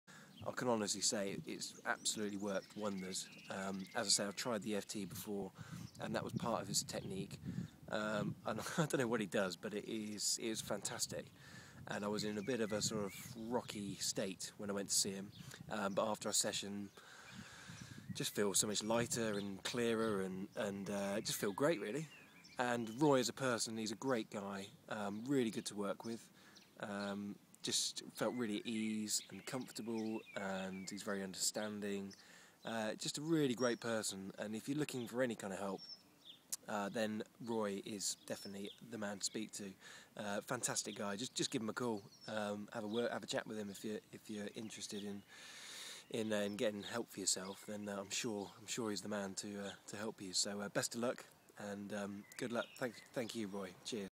Joe's Testimonial
Joe Frances, director for Gardens For Good, talks about his experience with Roy Martin.